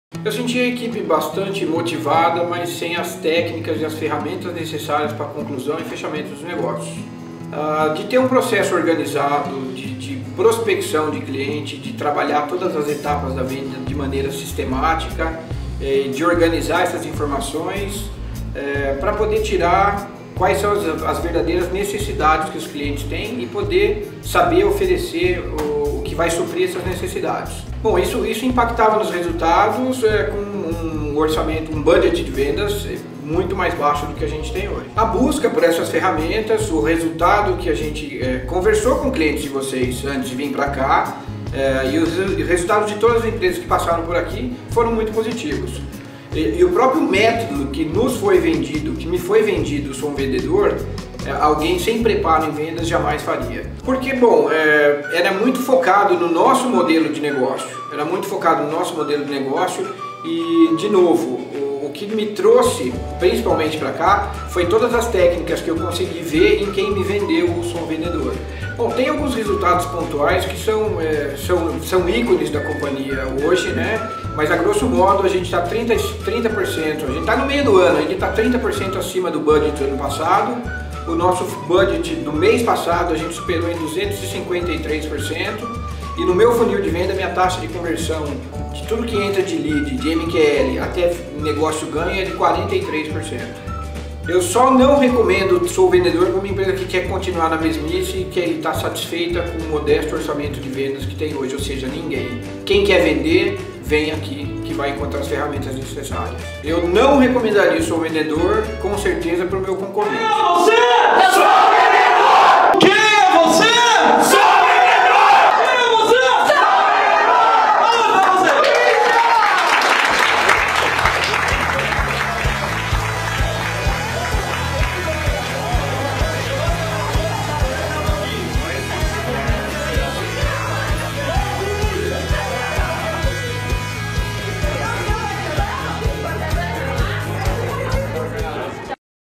A Startup Agrícola que Vendeu 253% A Mais Depois do
A InCeres é uma empresa de desenvolvimento de softwares para manejo da agricultura, que nasceu como resposta a uma série de problemas e dificuldades que as consultorias agronômicas enfrentavam ao atender seus clientes de agricultura de precisão. Estão participando da 9ª Turma do SOUVENDEDOR em Rio Claro e os resultados com certeza apareceram.